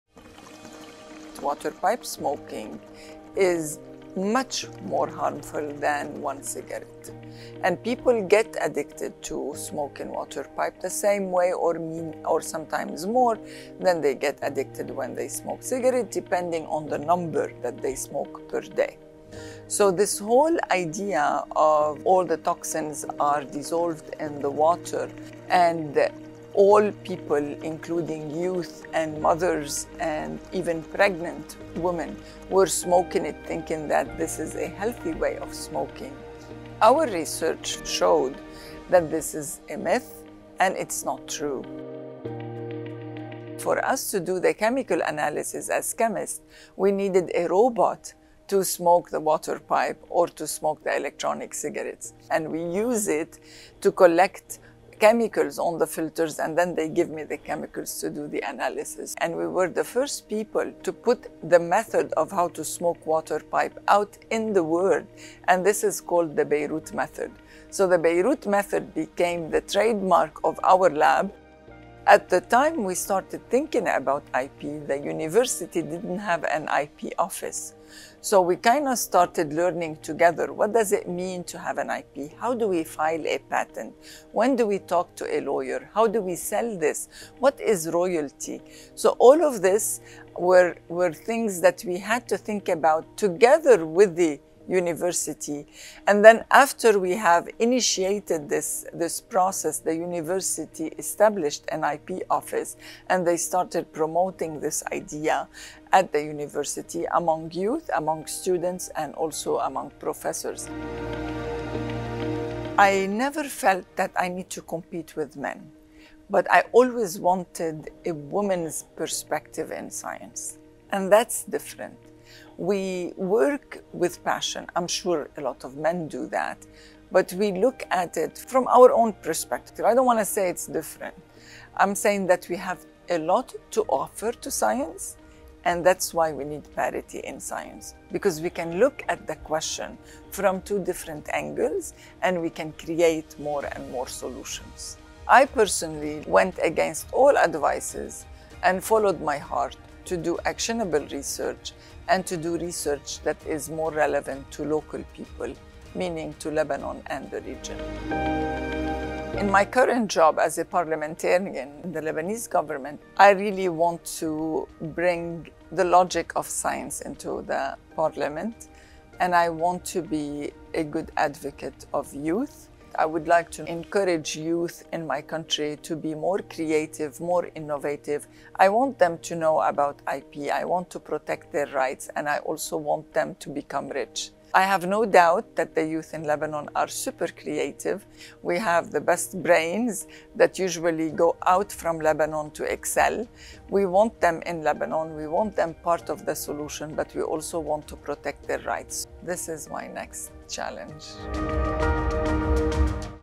Women in Science: Najat Aoun Saliba, Award-Winning Professor at American University of Beirut
The world needs science, and science needs women.

Professor Najat A. Saliba is a scientist and pioneering researcher on the effects of water pipe smoking on health.

Elected to the Lebanese parliament in 2022, Najat is also a laureate of the L’Oréal-UNESCO For Women in Science program.

In this video interview, Najat talks about her research, the importance of intellectual property for scientists, and her youth advocacy.

Along with other award-winning scientists, she participated in WIPO’s four-day Leadership Course on Intellectual Property, Science and Innovation for Women Scientists and Innovators. WIPO co-organizes the course annually in cooperation with UNESCO and the Korean Intellectual Property Office.